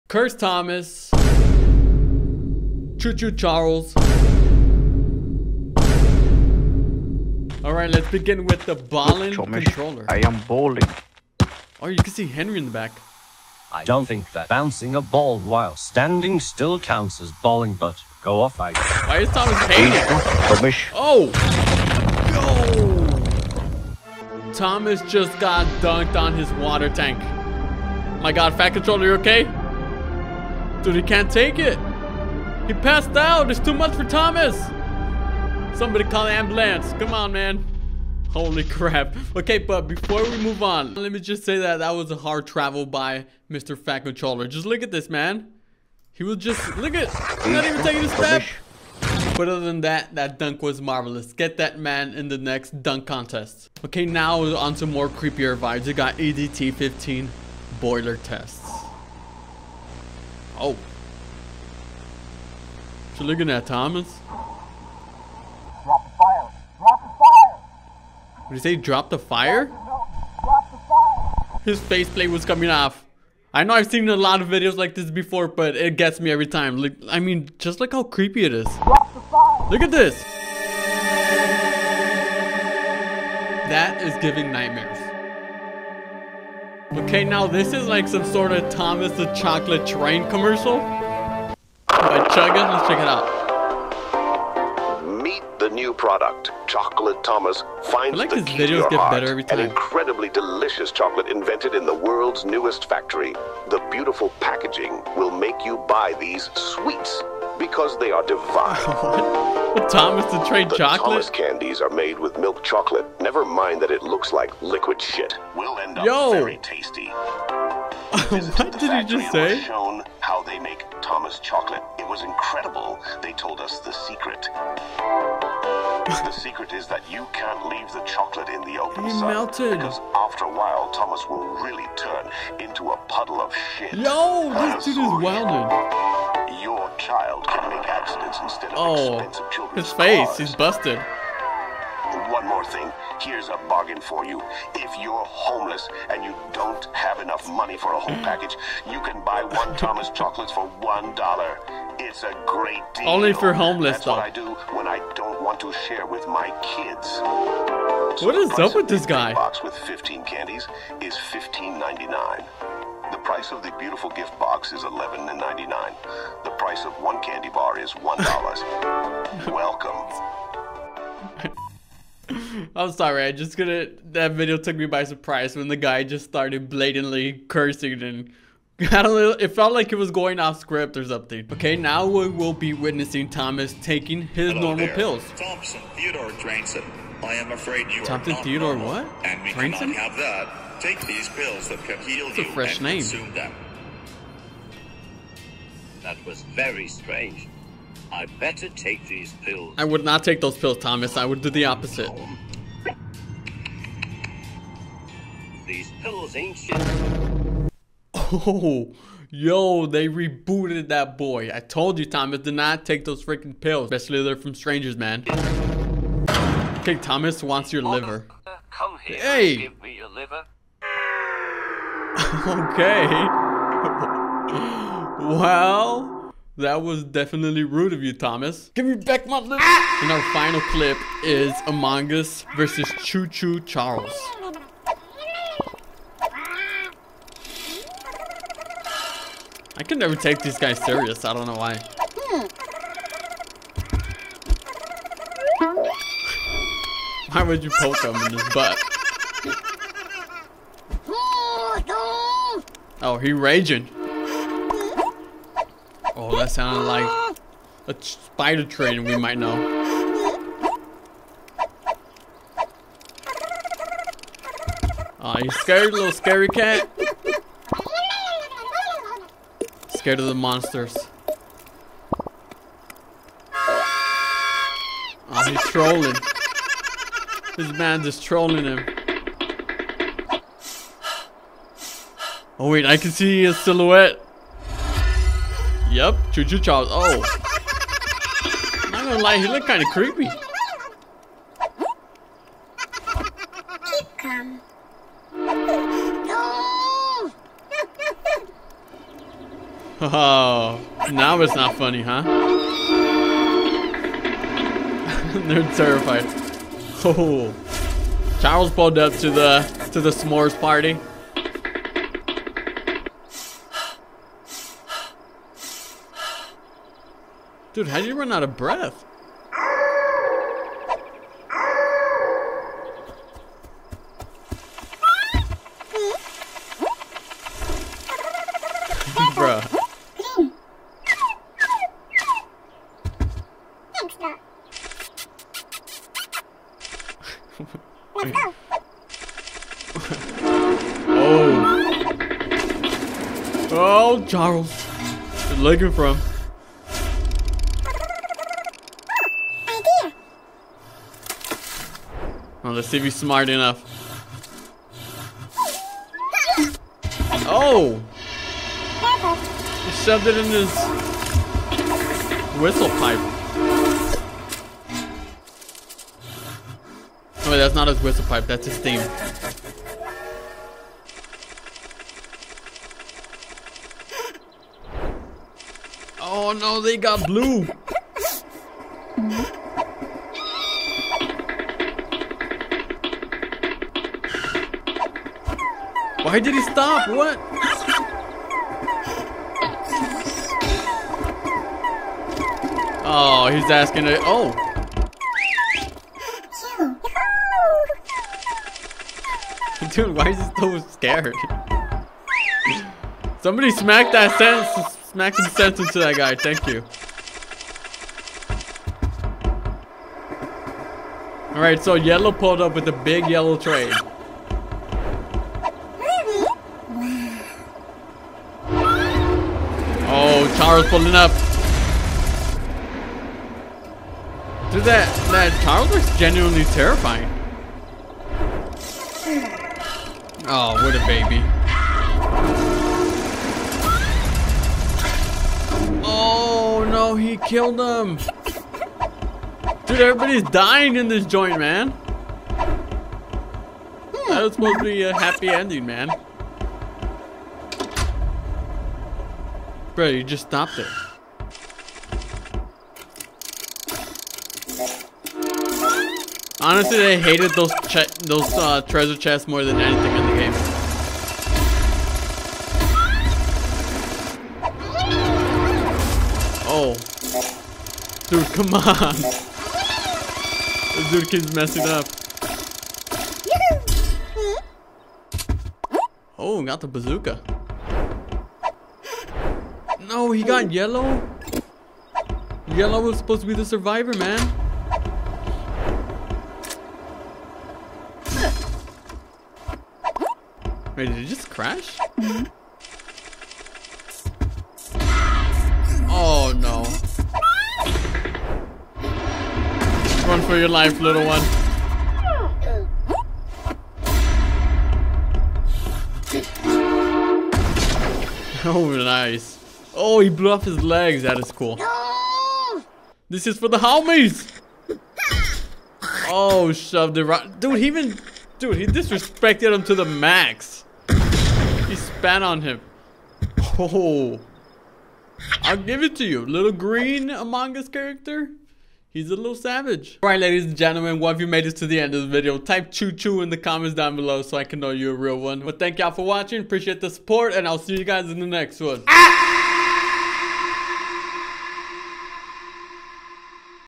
Cursed Thomas The Train.EXE VS Choo Choo Charles | Spider Train Animations
The evolution of Cursed Thomas vs Choo Choo Charles